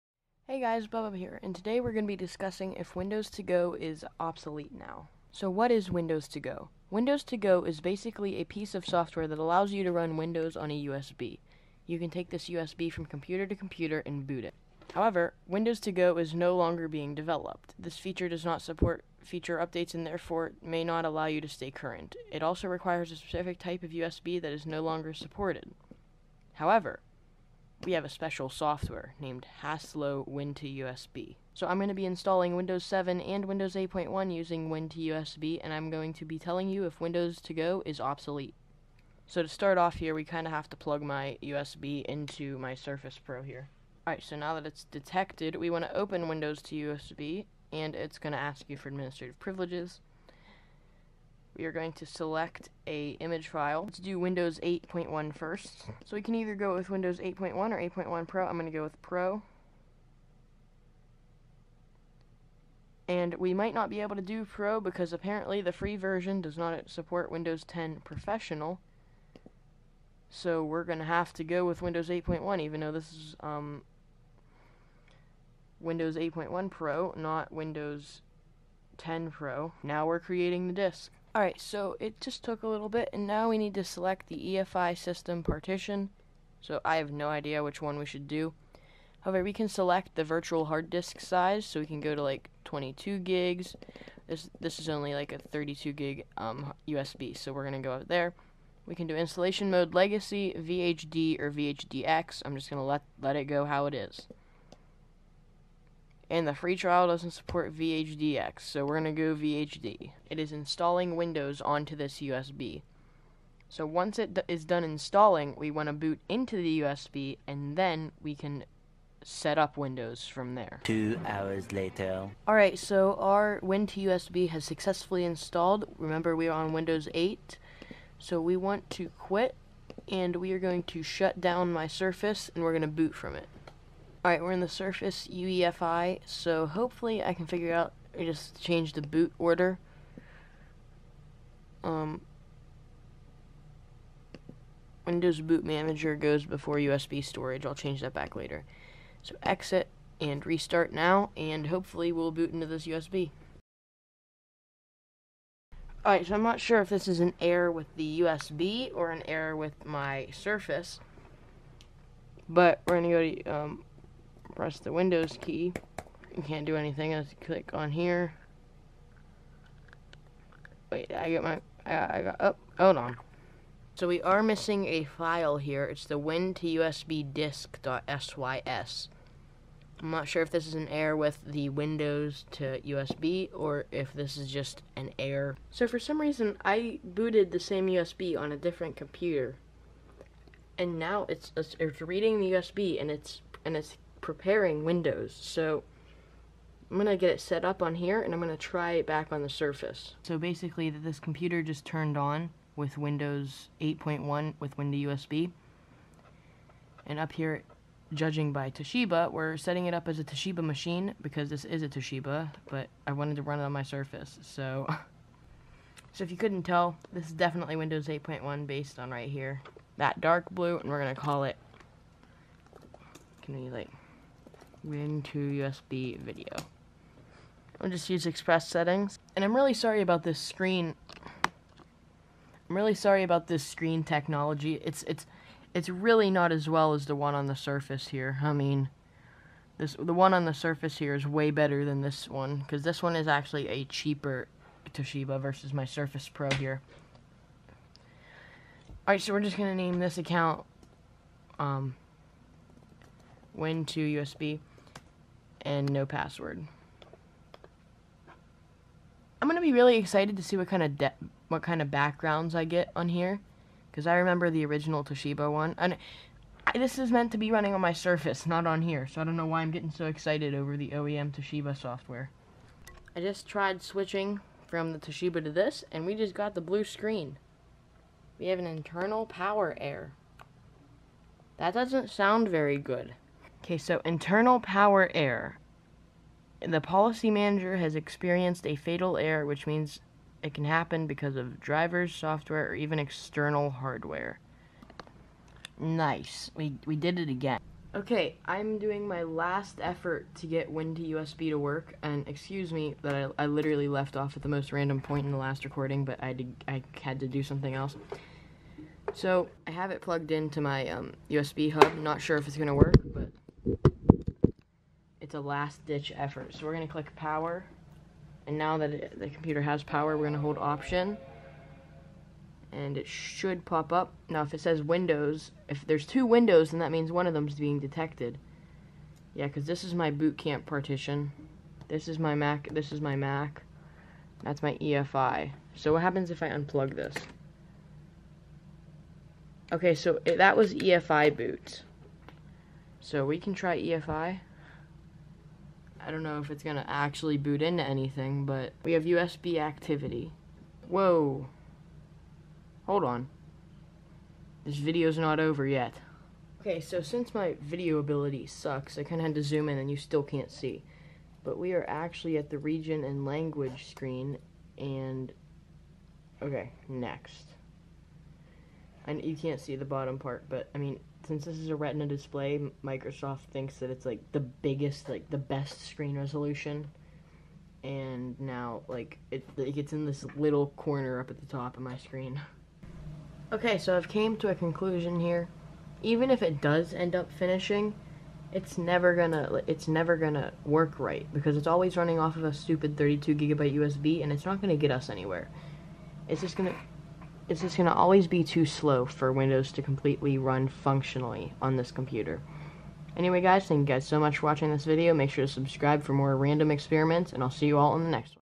Windows To Go - Is It Obsolete?
Today we discuss if Windows To Go is Obsolete in 2019!

►THINGS I DO ON MY CHANNEL:

Play Minecraft Hypixel (mc.hypixel.net)
Play Minecraft Single Player (Lets Play, EE2 Lets Play, Killing Enderdragon)
Vlog
Have fun!